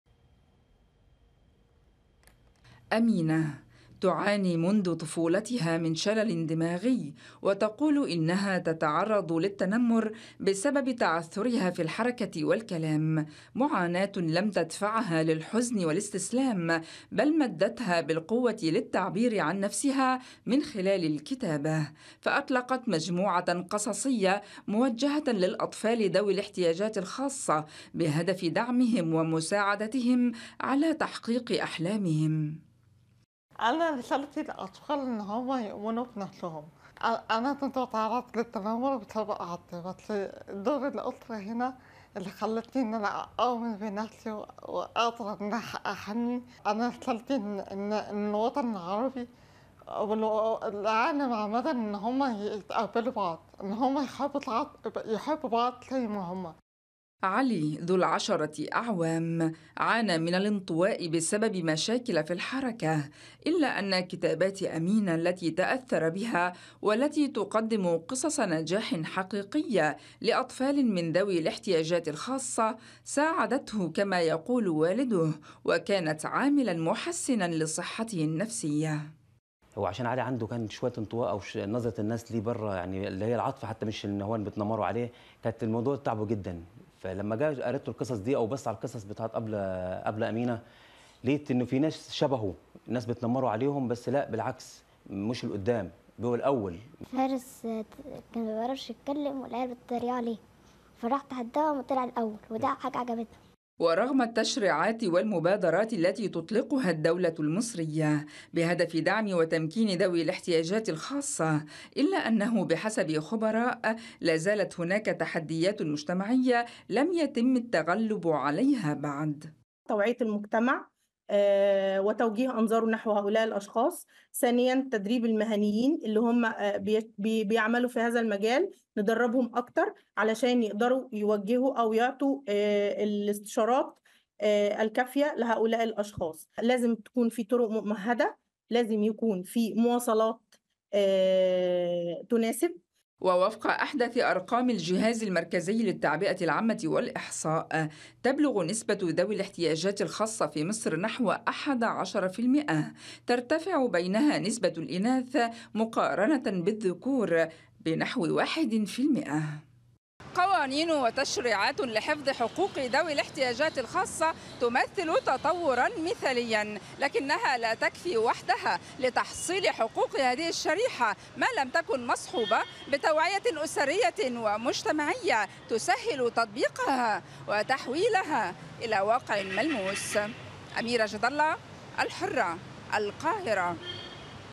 تعرف على "حكايات أبلة أمينة".. قصص أبطالها من ذوي الاحتياجات
ي مصر.. استطاعت "أمينة عبد الرحمن"، رغم إعاقتها وتعرضها للتنمر بسبب معاناتها من الإصابة بالشلل الدماغي منذ طفولتها، استطاعت مواجهة التحديات المجتمعية، من خلال إطلاق سلسلة قصصية بعنوان "حكايات أبلة أمينة".

تعرض "أمينة"، من خلال هذه  السلسلة، قصص نجاح ملهمة للأطفال ذوي الاحتياجات الخاصة.

حمل تطبيق "الحرة كونيكت" للتلفزيونات والهواتف الذكية:

أمازون فاير